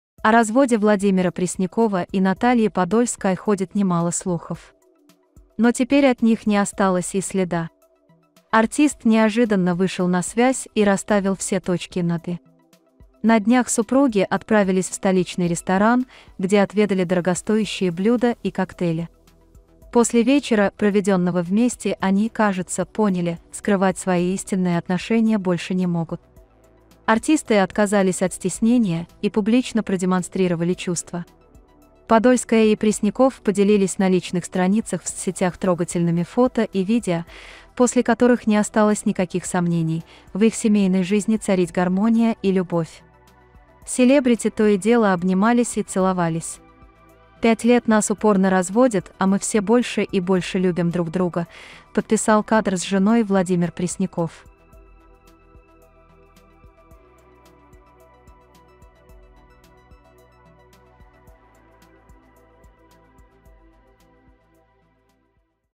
Публичное признание: Пресняков одной фразой выдал правду о разводе с Подольской
Певец показал, что на самом деле происходит в паре.